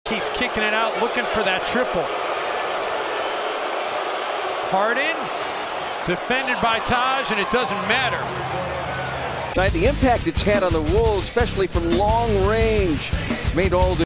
NBA, playoff 2018, Rockets vs. Timberwolves, Round 1, Game 3, Move 8, James Harden, 3 pointer
Houston Rockets - Minnesota Timberwolves, Game 3, 21/04/2018, quarter 1
James Harden, 3 pointer